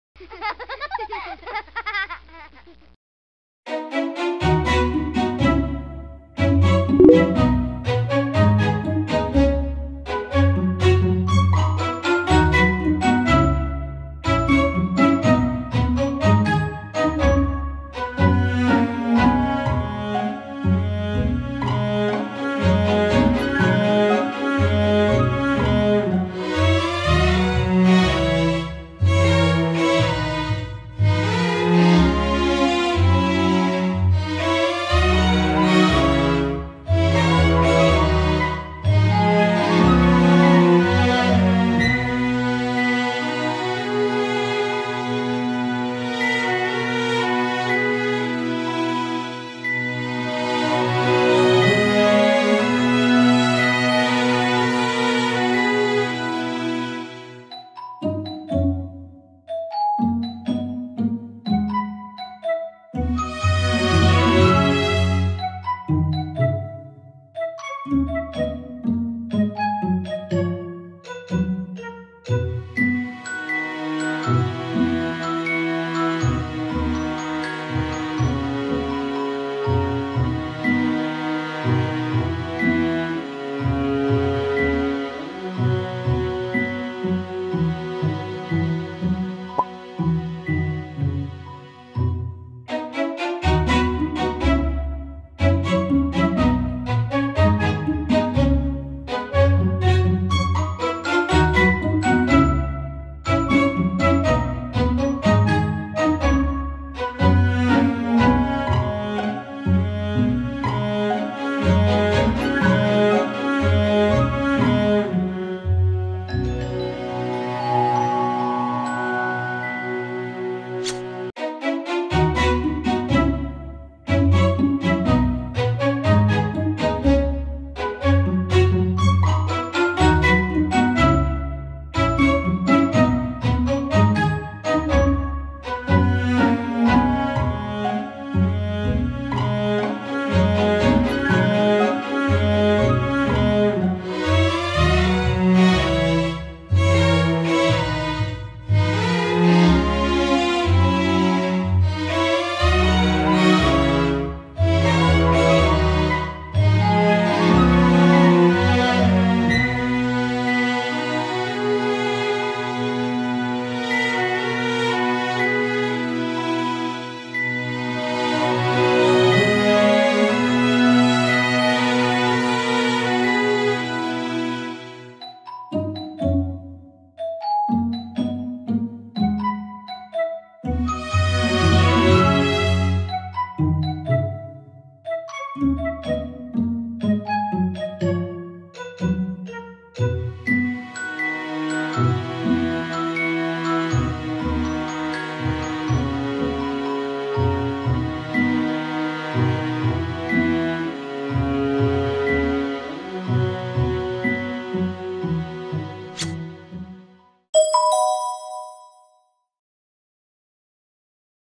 Coloring Maya and Willy from Maya the Bee Movie | Ohuhu Art Markers
Today we are coloring Willy giving Maya a sweet kiss on the cheek. How cute! 🐝 💖